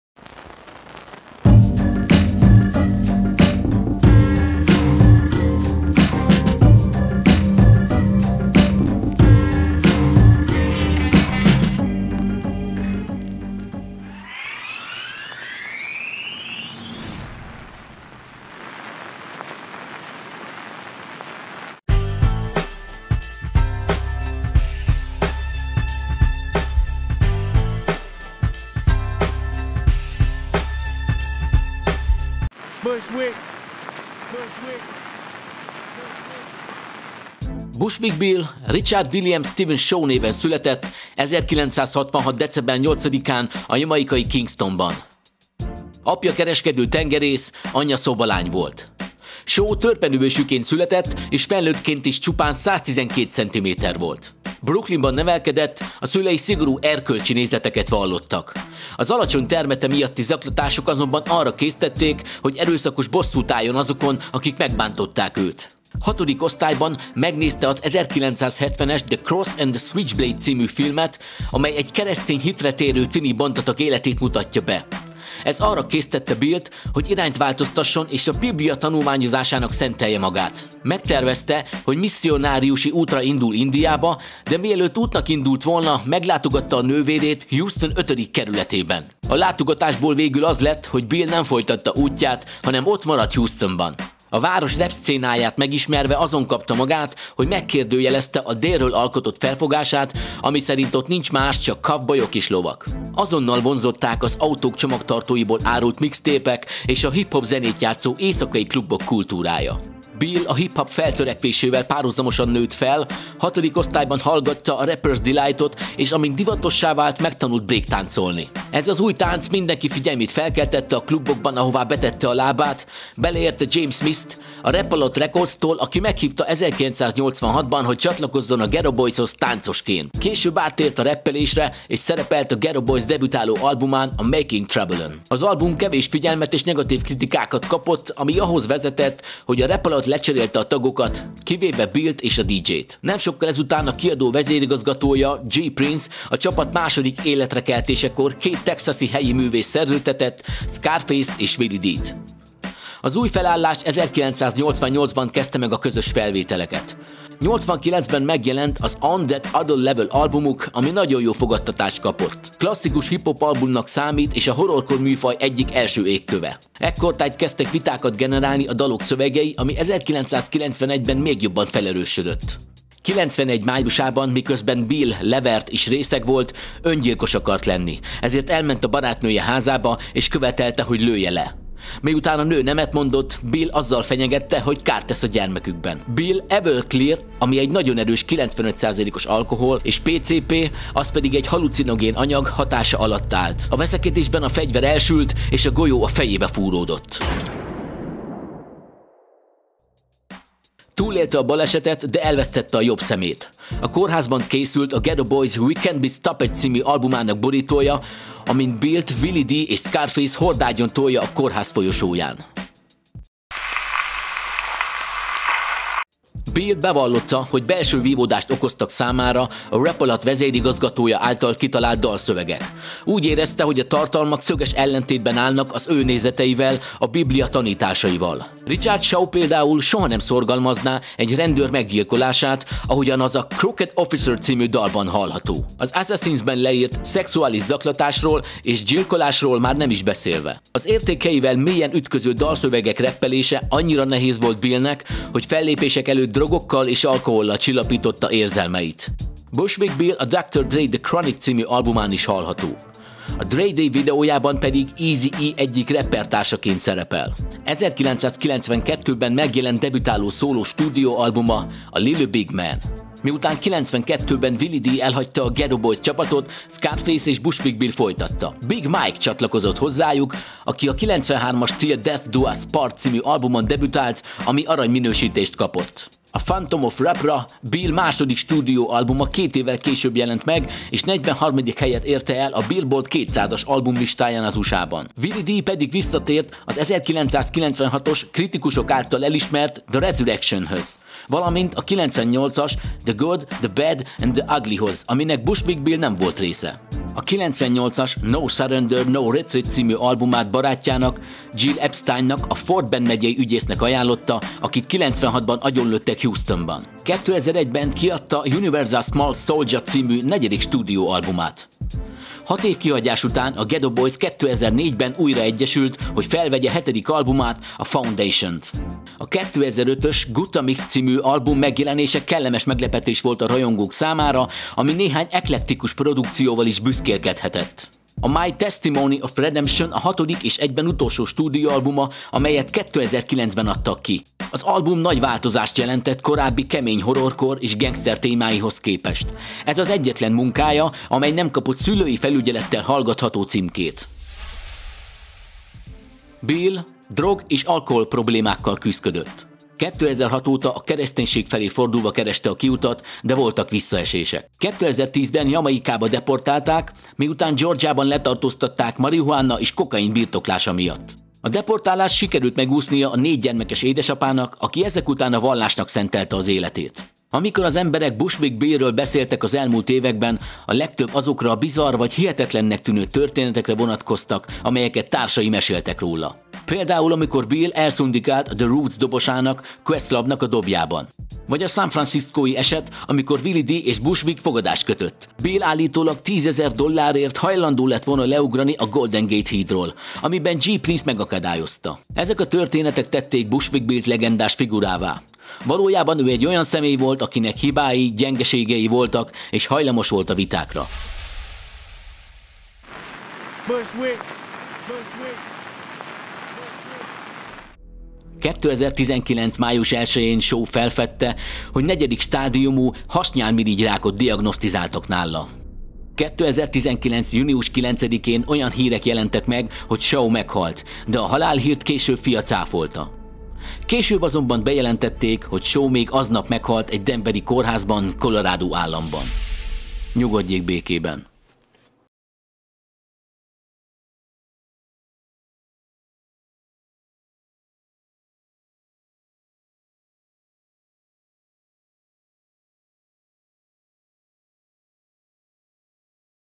BUSHWICK BILL - A Kis Geto Boy / A Golden Age Alakjai
Az okos gyerek bemutatja:
A Golden Age Alakjai című videó sorozatot,
ami bemutatja 90-es évek Rap előadóit.

Narrátor : Az okos gyerek / Jossa
Szöveg és szerkesztés : Az Okos Gyerek és  DFA
Video : Az okos gyerek
Aláfestő zene : JOSSABEATZ a.k.a. Az okos gyerek